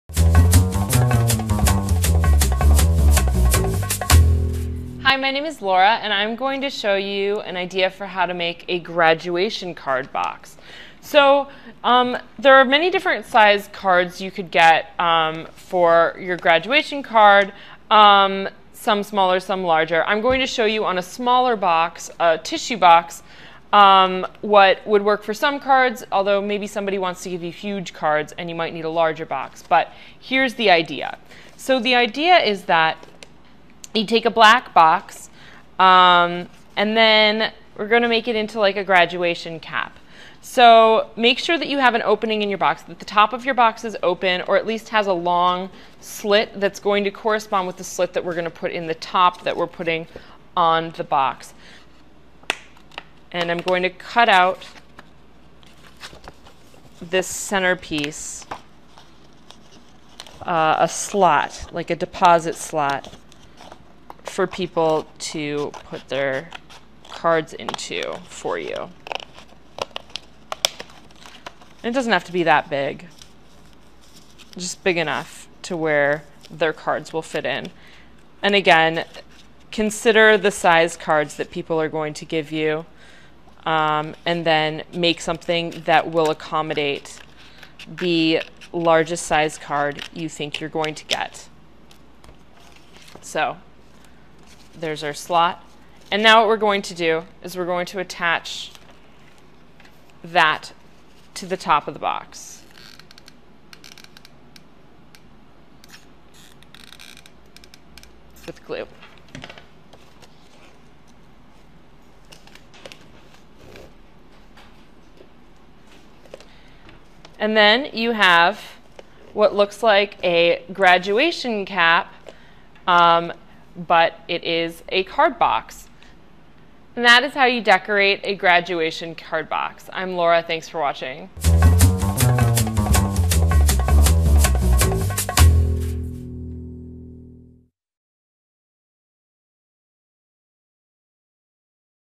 How to Decorate a Graduation Card Box
How to Decorate a Graduation Card Box. Part of the series: Arts & Crafts. The size of a graduation box will depend largely on the situation and will require a tissue box big enough to meet demand. Decorate a graduation card box with help from an expert with an extensive background in drawing, including a college education at Columbia in Chicago in this free video clip.